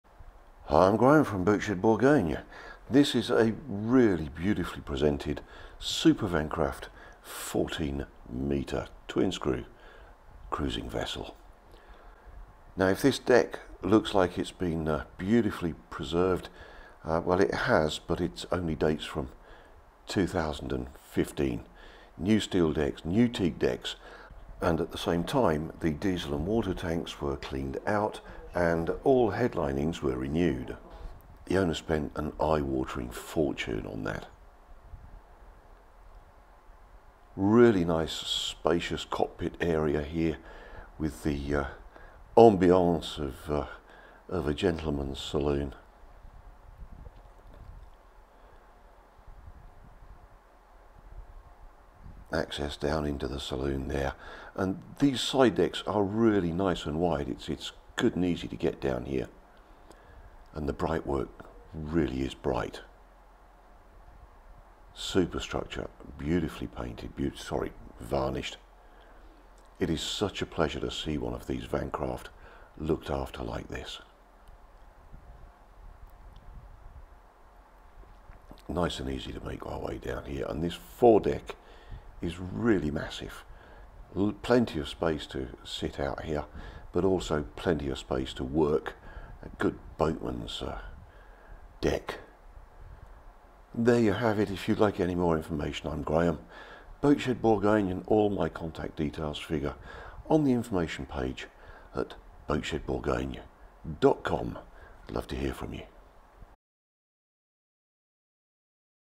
SUPER VAN CRAFT 1400 Extensive refit 2015 - Boatshed - Boat Ref#323773
SUPER VAN CRAFT 1400 Extensive refit 2015 for sale with Boatshed Bourgogne Photos and video taken by Boatshed Bourgogne